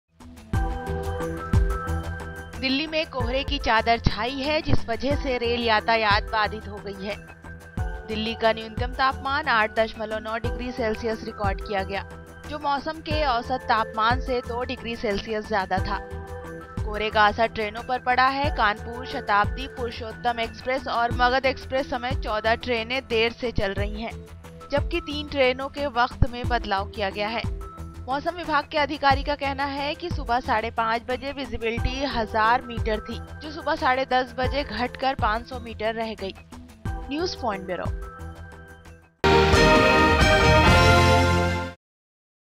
Fog engulfs Delhi, 12 trains run late : NewspointTV
According to Met office, the minimum temperature in the city settled at 9.0 degrees Celsius. At least 12 trains, including the Grand Trunk (GT) Express and Howrah-Duronto Express, were running late, a statement from the Northern Railway said.

The Met office, however, forecast a clear day ahead. "The sky will be mainly clear. Moderate fog is likely to occur at most places from morning to forenoon," an official from the India Meteorological Department said.